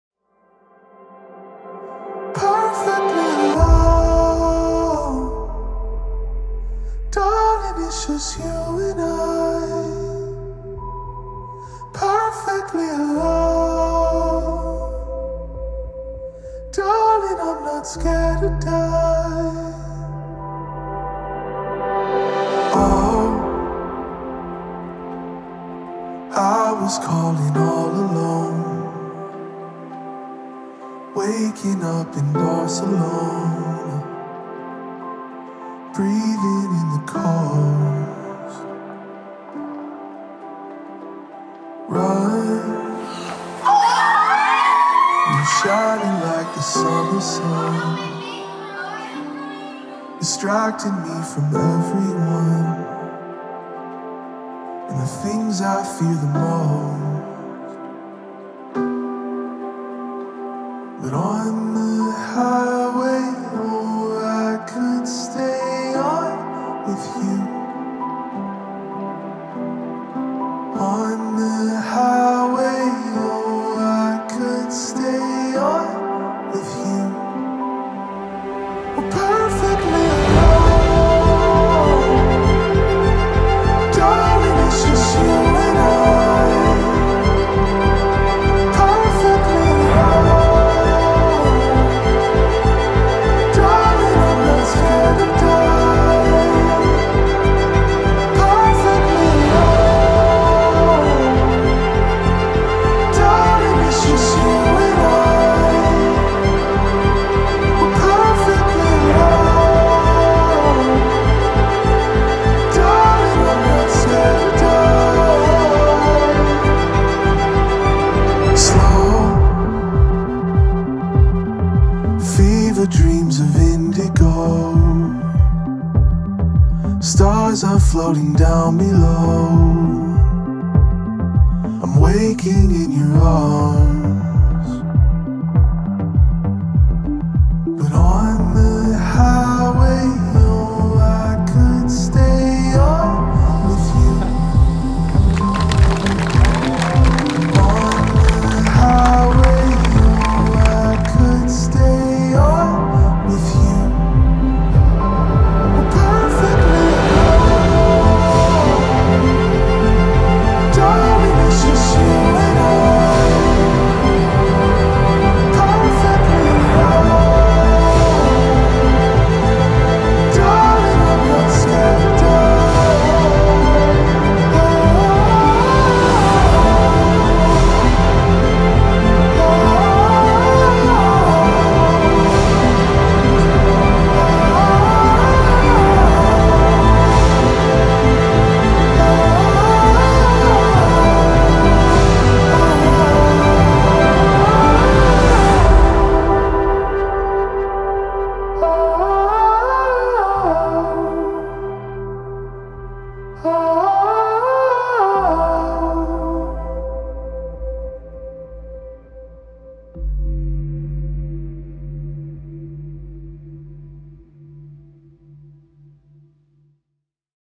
Mckena and Newlon Wedding Highlight Film
The sweetest late summer wedding at Houmas House Plantation in Darrow, Louisiana